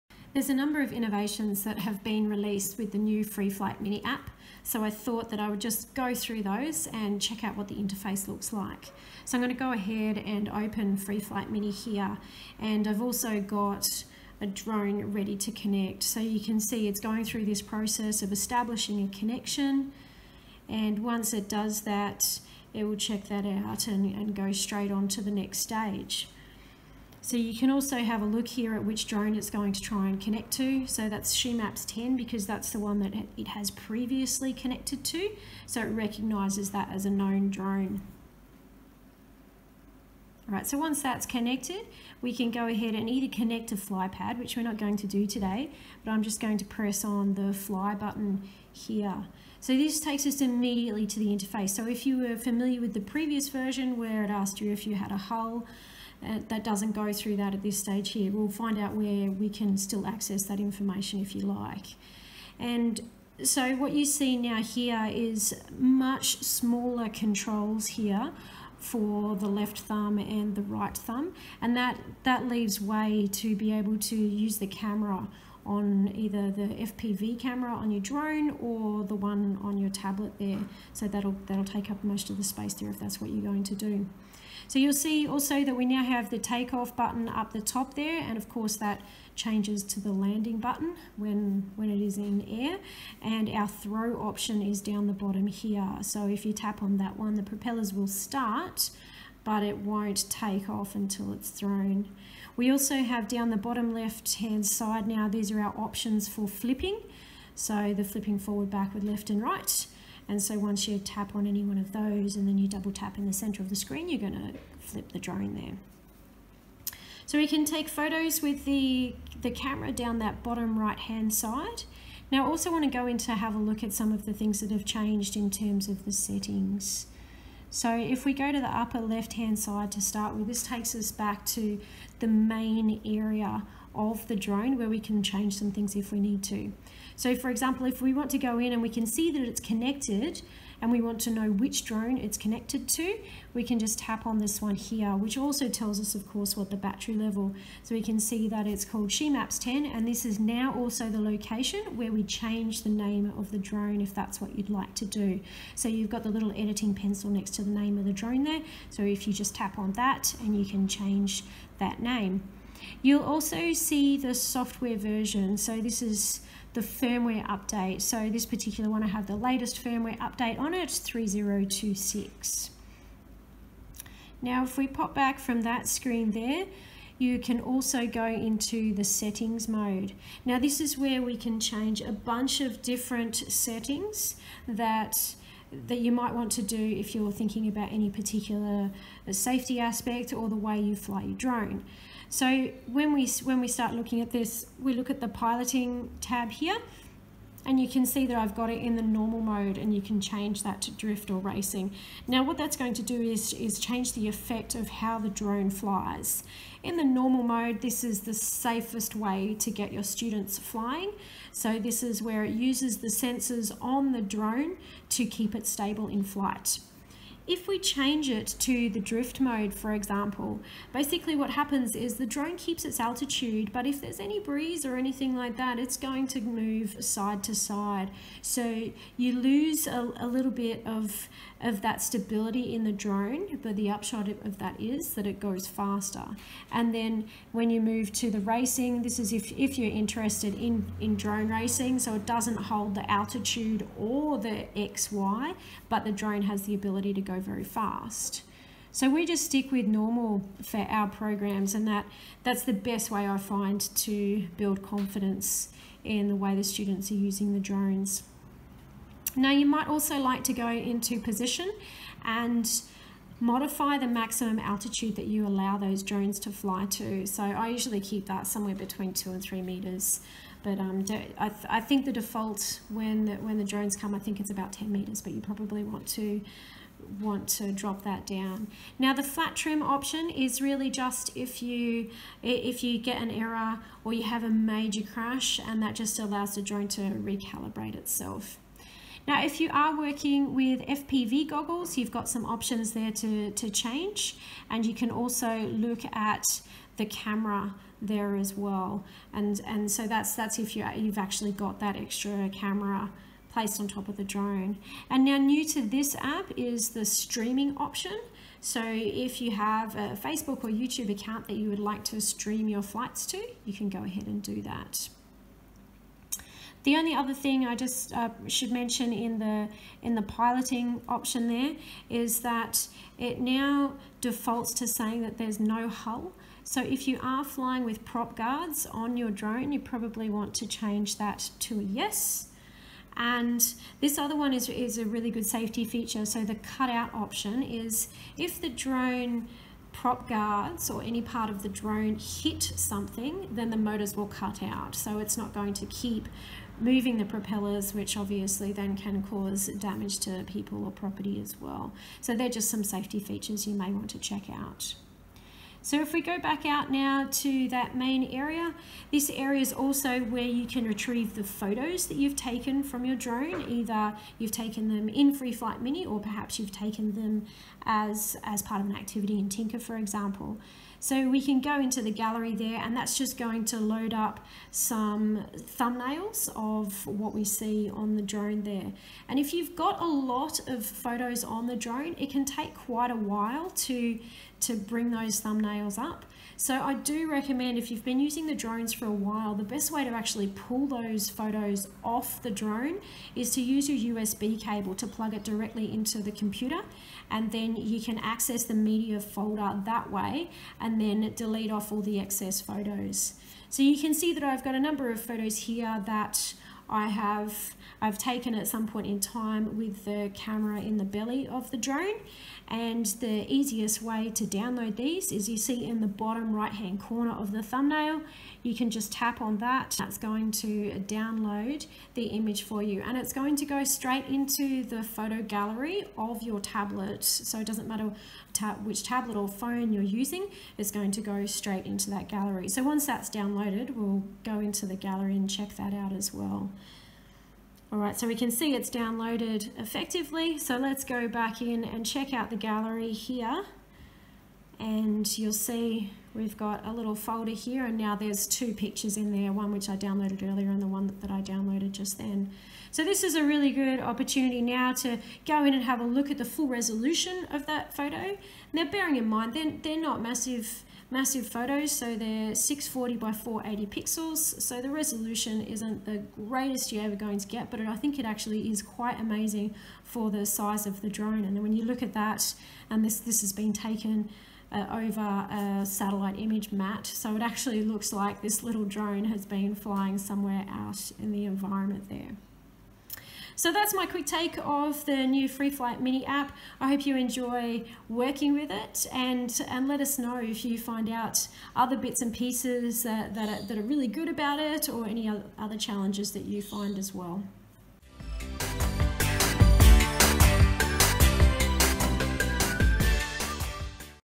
A look into the new Parrot Free Flight Mini App
Parrot has just done a significant upgrade to its Free Flight Mini App for iOS and Android. This is the app used to pilot the Mambo and other Parrot Minidrones. I take a look at some of the new and changed features here.